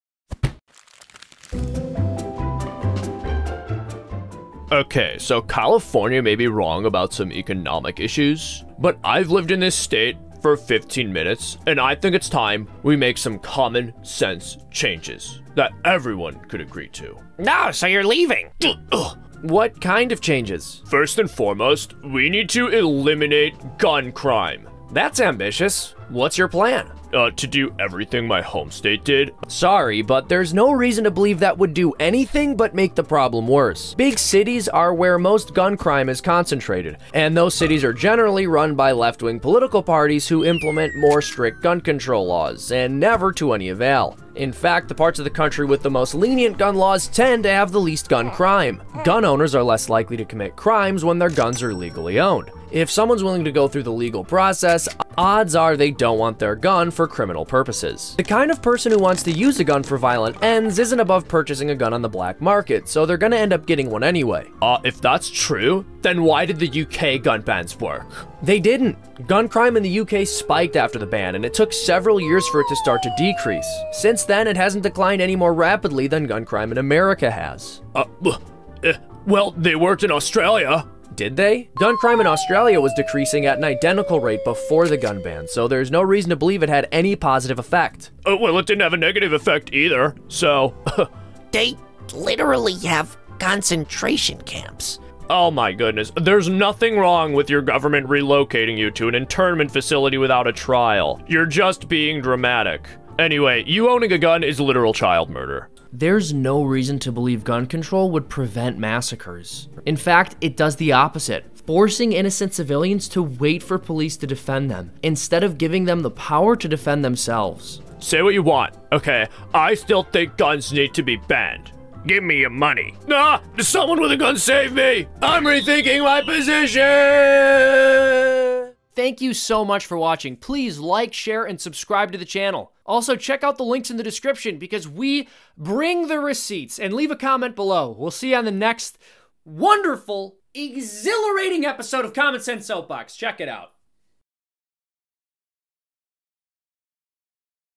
There's Nothing "Common Sense" about Gun Control
On this episode of Common Sense Soapbox, Bubble Man argues for “common sense” gun control to cut back gun violence. The only problem is, there’s no evidence to suggest his proposals would solve the issue.
CREDITS:
Written by Sean W. Malone & Seamus Coughlin